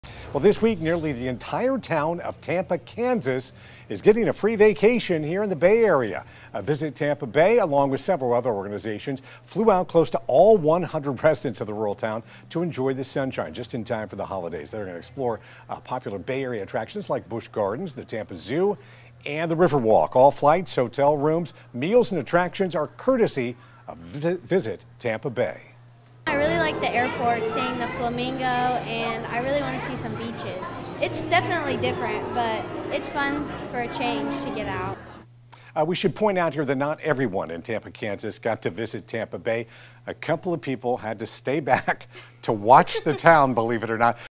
Group of visitors traveling from Tampa to... Tampa?
From one Tampa to another, a group of Tampa locals will be traveling all the way from Kansas to Florida.

That's right, they're traveling from Tampa, but not "our" Tampa.

Ninety-five people will be in the local area as they arrive from Tampa, Kansas, Tampa International Airport said in a tweet.